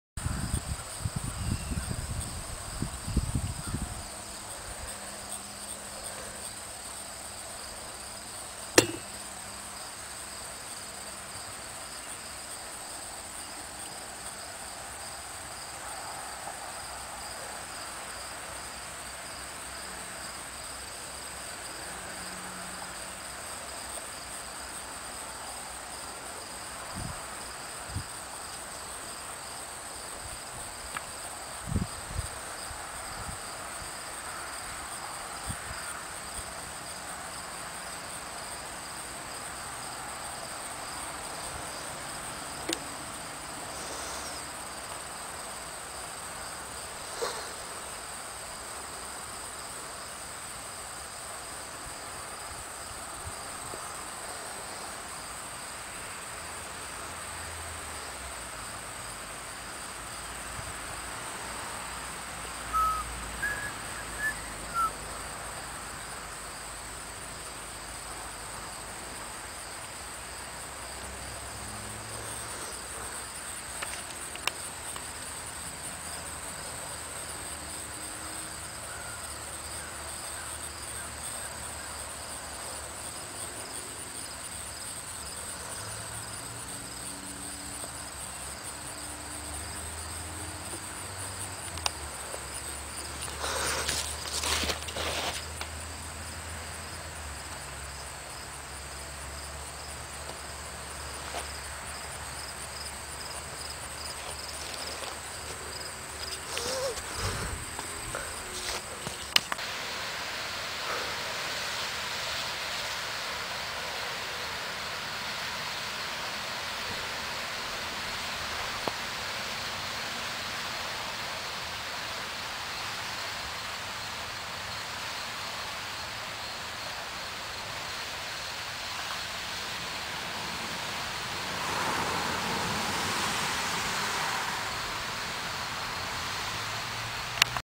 Squirrel and butterfly vid
i was bored and took a vid of a butterfly then a squirrel Xd